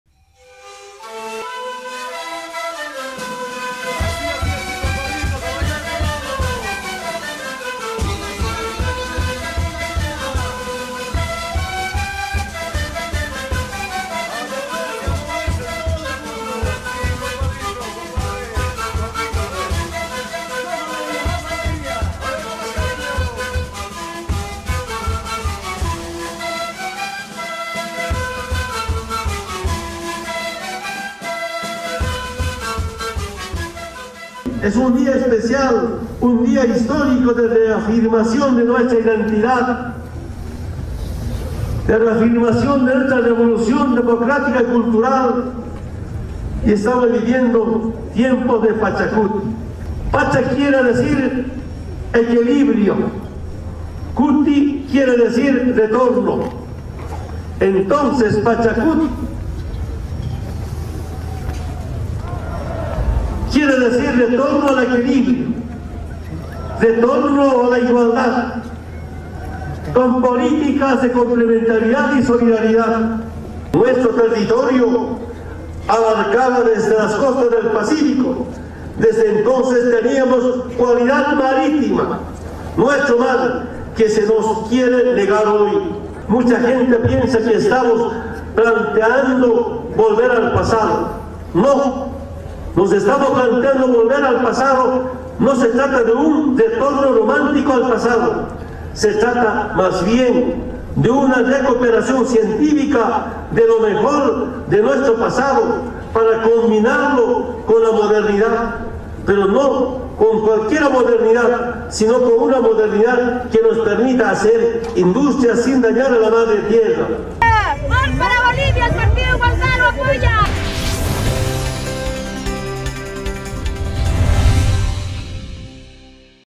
Evo Morales instó durante su asunción a cuidar los recursos que brinda la tierra
El presidente de Bolivia, Evo Morales, abogó hoy, en su asunción ante la milenaria cultura indígena aimara representada en el templo de Tiahuanaco, por "frenar la loca carrera de la destrucción de la Tierra en nombre del desarrollo", y reivindicó el "vivir bien" de su gobierno sobre el liberalismo o socialismo europeo que "pasó a la historia junto a la república liberal y colonizadora de Bolivia".

Al acto concurrieron representantes de 40 países, entre ellos una delegación de alto nivel de Estados Unidos, nación con la que están rotas las relaciones diplomáticas a nivel de embajadores desde 2008.

Chile, con quien las relaciones están rotas desde hace más tiempo por el litigio por la salida al mar que reclama Bolivia, envió sólo una representación del Senado.

Al igual que al asumir el cargo en sus primeros dos mandatos, en 2006 y 2010, esta celebración ante el mundo indígena se realizó en la víspera de la asunción formal de la Presidencia, en el que será su tercer mandato consecutivo, ante el Legislativo boliviano en La Paz.

Morales que ratificó la necesidad del "retorno al equilibrio" entre cuidado del medio ambiente y el desarrollo, la afirmación de la identidad indígena y de la "revolución democrática y cultural" de su gobierno, dijo que "no planteamos volver al pasado. Se trata de una recuperación científica de lo mejor de nuestro pasado para combinarlo con la modernidad".

Las ceremonias de esta investidura indígena comenzaron con una "limpieza" del gobernante efectuada por sabios de la cultura aimara, que lo acompañaron por los lugares más simbólicos del templo preincaico de Tiahuanaco y luego recibió el bastón de mando como punto de partida de un nuevo período de gobierno (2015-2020) con la energía de las divinidades ancestrales de la Pachamama y el cosmos.

Ante miles personas que asistieron al ritual, entre representantes de organizaciones sociales bolivianas y extranjeras, Morales hizo referencia al nacimiento de Bolivia en 1825 como un país con litoral, cualidad que perdió tras una invasión chilena a finales del siglo XIX, y por la que inició una demanda en la Corte Internacional de Justicia (CIJ) de La Haya.

"Nuestro territorio abarcaba desde las costas del Pacífico, desde entonces teníamos cualidad marítima, nuestro mar que se nos quiere negar hoy", subrayó, citado por la agencia estatal ABI.

Luego remarcó que este "Es un día especial, histórico, de reafirmación de nuestra identidad, de reafirmación de nuestra revolución democrática cultural. Estamos viviendo tiempos del Pachakuti, pacha quiere decir equilibrio, kuti quiere decir retorno, entonces Pachakuti quiere decir retorno al equilibrio, retorno a la igualdad con políticas de solidaridad y complementariedad para este proceso de cambio".

A juicio de Morales, "es tiempo de consolidar la revolución educativa y cultural, la revolución de la salud, la revolución en la producción, en la justicia en el trabajo, la revolución del pensamiento".

"Estamos en el tiempo de un gran parto, parto de la esperanza, de la unidad, de la armonía, de la felicidad y de nuestra filosofía de vida", subrayó.

Afirmó que ese ritual ancestral que se realizó en el centro de Tiahuanaco es un acto milenario y sagrado para la vida y para la humanidad después que los pueblos indígenas soportaron por más de 500 años "la obscuridad, el odio, el racismo, la discriminación y el individualismo", tras la llegada de los "hombres extraños" que con su filosofía de muerte intentaron hacer desaparecer a los pueblos indígenas del mundo.

"Nos dijeron que había que modernizarse, había que civilizarse, y para eso nos enseñaron su filosofía de muerte, pero para modernizarnos, para civilizarnos, primero tenían que hacer desaparecer a los pueblos indígenas del mundo, hacer desaparecer nuestro idioma, nuestra cultura, nuestras raíces, nuestra hoja de coca, nuestros productos naturales, como la quinua, nuestros ganados, nuestros símbolos, nuestra música, nuestra vestimenta, querían terminar con nuestra identidad", enfatizó.

El presidente boliviano afirmó que de esa filosofía de muerte nació el esclavismo, el capitalismo y el imperialismo, y aseguró que para que exista el capitalismo, el imperialismo, "tenían que hacer desaparecer a los pueblos indígenas del mundo".

Pero aseveró que los pueblos indígenas sabían que "nuestro tiempo volvería" y afirmó que lucharon permanentemente para comandar su destino.

"Hemos vuelto, no pudieron hacernos desaparecer, estamos para gobernar nosotros mismos", subrayó al parafrasear a Túpac Katari, líder indígena que se rebeló en 1871 contra los colonizadores y antes de ser descuartizado por cuatro caballo dijo: " volveré y seré millones".